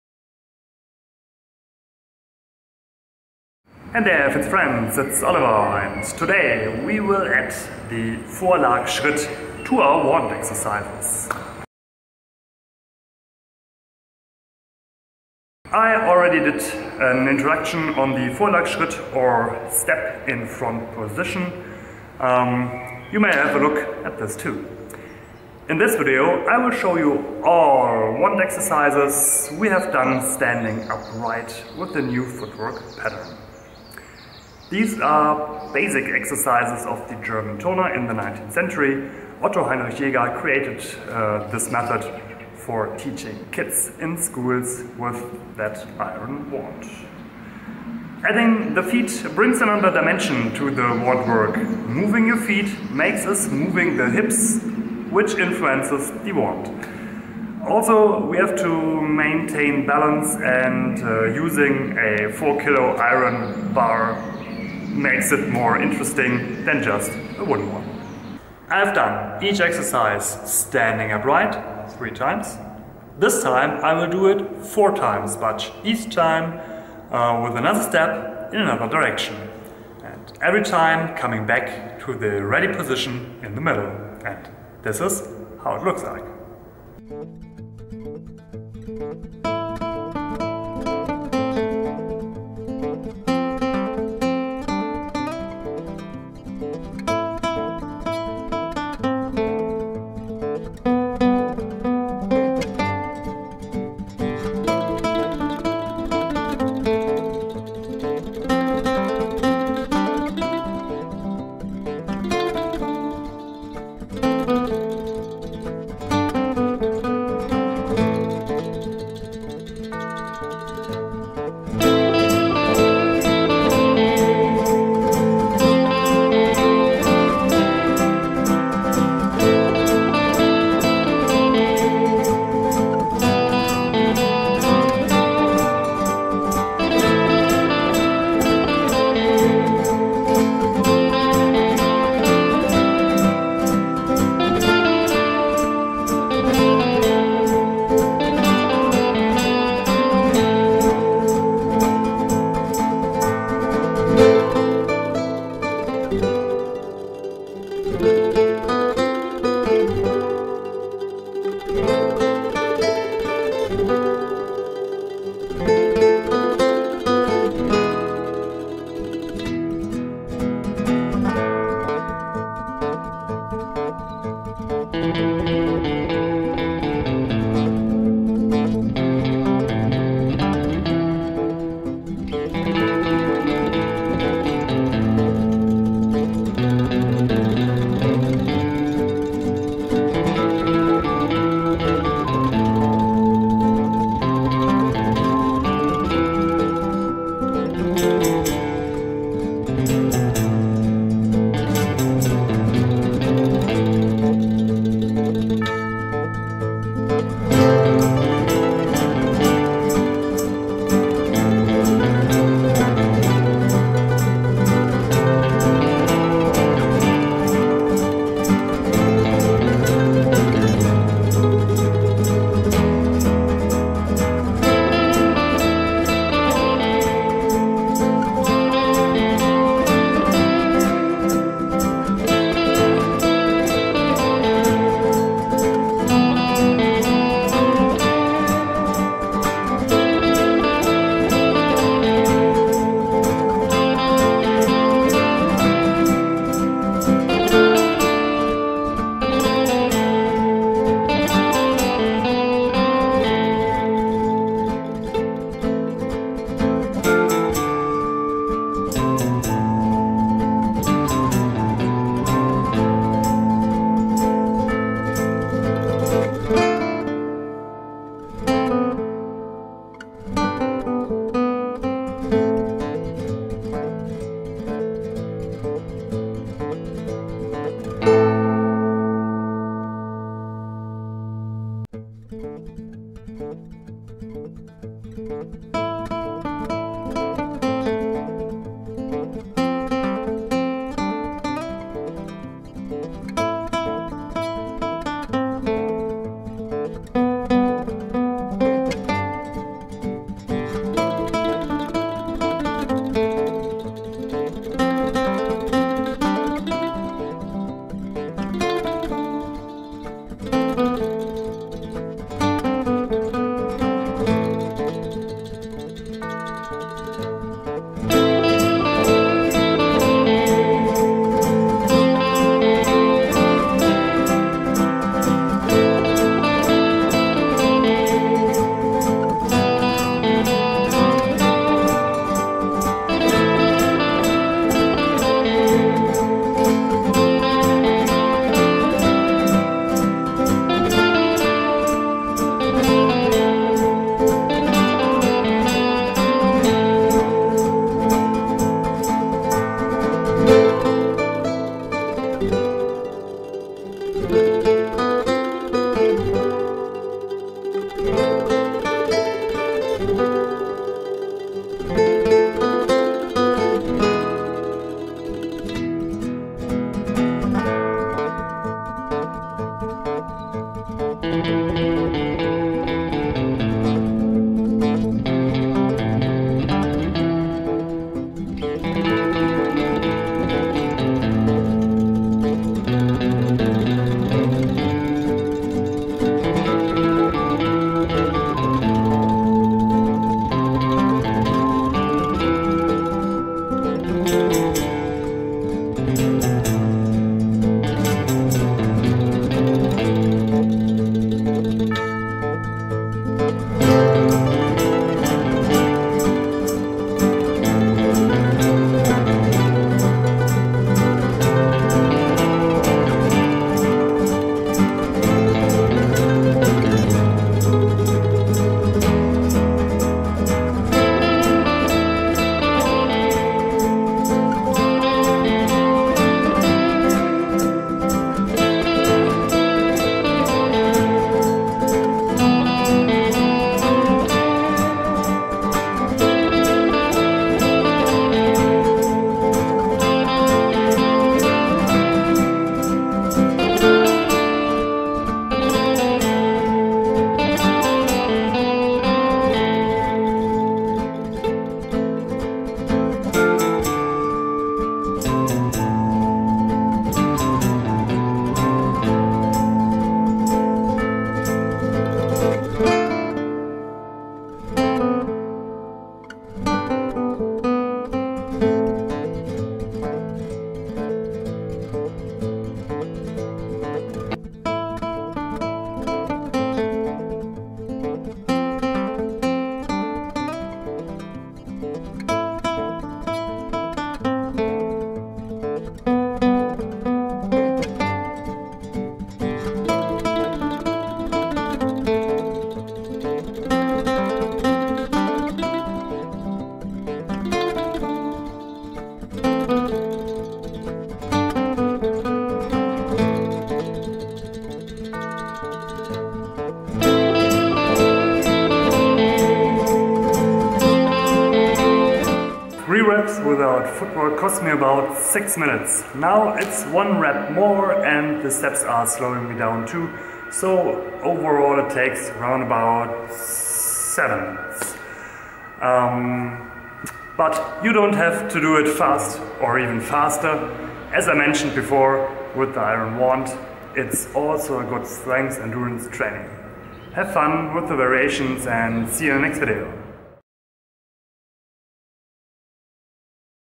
05 The Vorlag with ALL original old school iron wand exercises from the German Turner
Old school iron wand exercises of the Turner. These kind of physical education were strength and stretching exercises in once. Today also called stick mobility.

Today 19th cent. gymnastics are sometimes called European yoga. The gymnastic exercises like calisthenics those days are also done with Indian club, dumbbell or wand. Today they are en vogue again and preached by some fitness Influencer.
Otto Heinrich Jäger did some exercises with weighted sticks for kids. Today these are also good exercises for us adults and not just physical education for children. Using iron wands instead of wood gives these exercises a strength endurance component.

0:00 Intro
1:27 Exercise 210
8:30 Outro